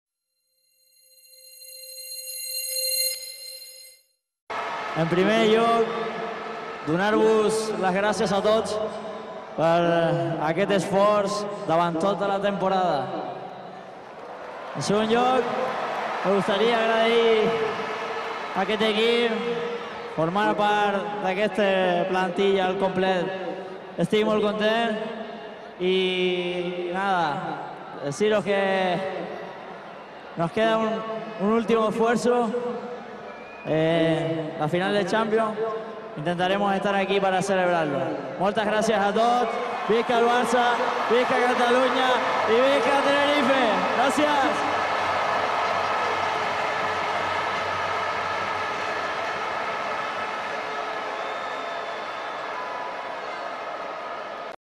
Pedro Rodriguez - FC Barcelona Celebration of La Liga 2010 - 2011 in the Camp Nou
Pedro Rodriguez in the Camp Nou.
"Thank you to everyone for all their efforts throughout the season. I'm happy to be part of this team and now we have one big effort to make -- the Champions League Final and we hope to bring it back here".